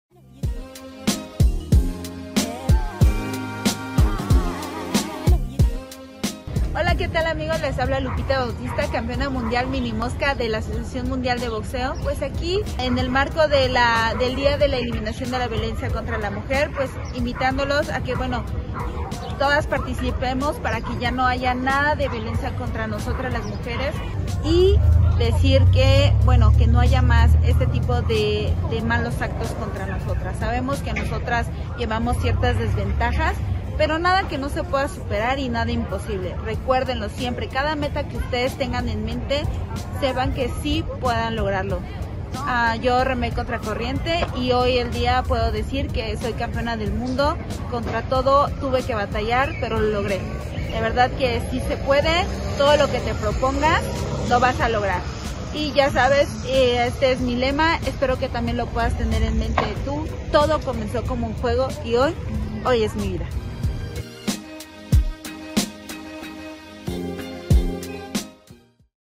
🟠 LUPITA BAUTISTA "LA LICENCIADA", UNA GRAN MUJER DE IZTAPALAPA 🟠
Lupita Bautista "La Licenciada" es oriunda de la SierraDeSantaCatarina y una orgullosa mujer de Iztapalapa; su historia de lucha y esfuerzo es un gran ejemplo para todas y todos, y nos demuestra como la pasión por el deporte es un motor para la transformación.

¡Por mujeres Siemprevivas, siempre libres y siempre seguras!